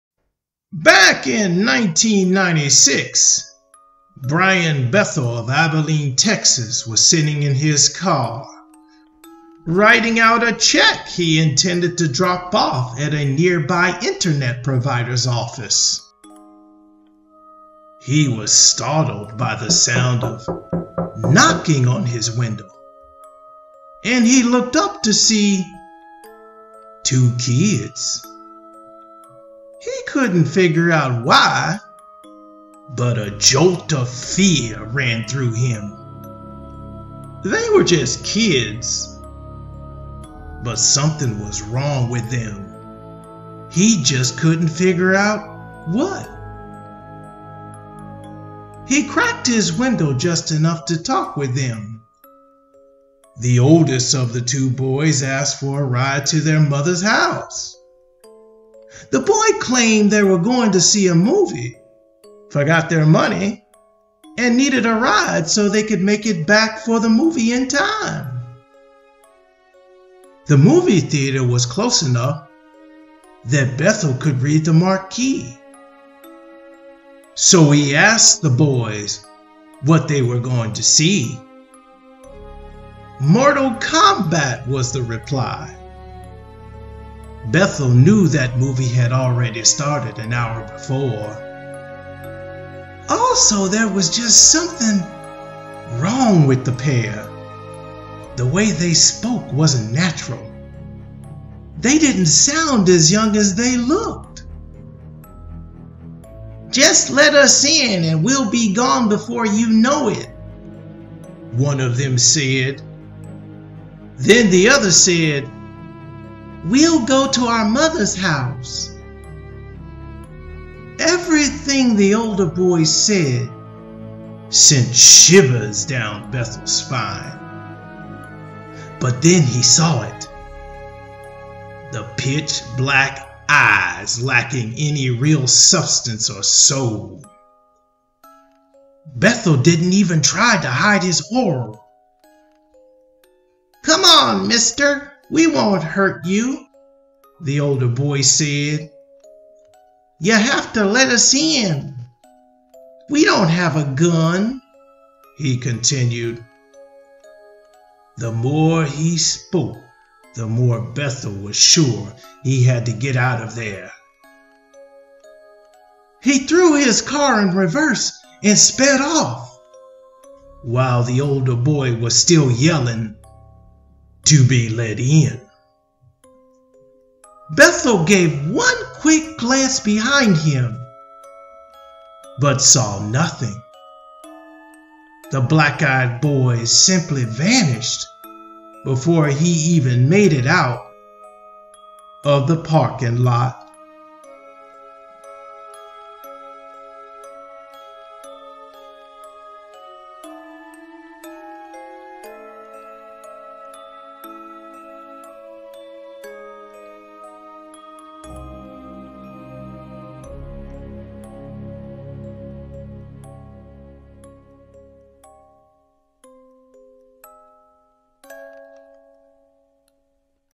Abilene's Darkest Secret: The REAL Story of Black Eyed Children
Abilene, a city in Taylor and Jones County, Texas, has a legendary children problem. Said problem was unforeseen by the cattlemen who established it as a stock shipping point on the Texas and Pacific Railway in 1881. Apparently, something on the former Chisholm Trail at the founding of this town got disturbed, stirring up something evil, affecting the children even to this day.

You are welcome to sit by the fireside and experience the strange behavior of the children yourself. But beware!